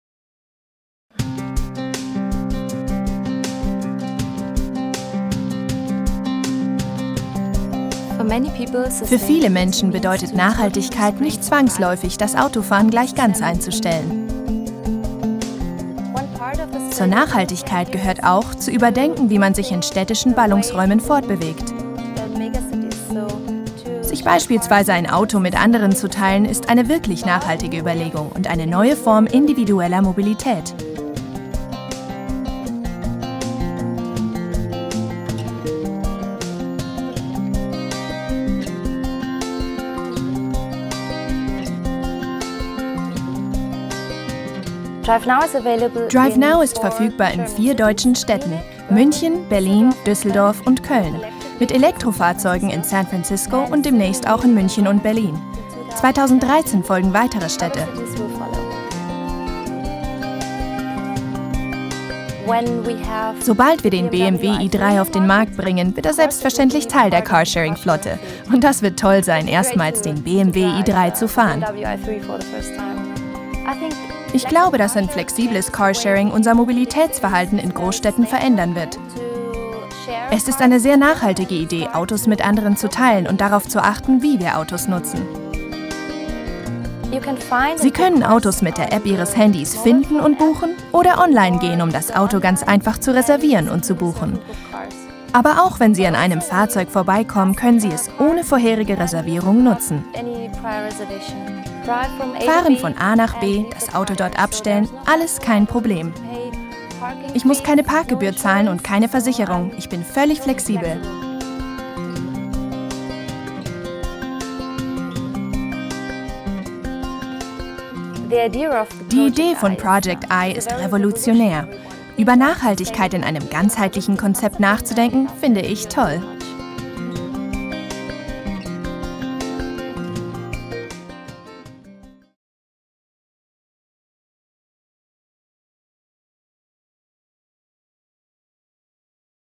BMW i: Nachhaltige Mobilität erleben. Auch ohne eigenes Auto. Car Sharing.
Flexibel und mobil -- ohne eigenes Fahrzeug: Car Sharing mit DriveNow bietet nachhaltige Lösungen für die individuelle Mobilität in der Großstadt. Motor einer neuen Mobilität Erfahren.

Mehr Infos zum Thema gab es im Rahmen der BMW i Innovationstage im April 2013 in Leipzig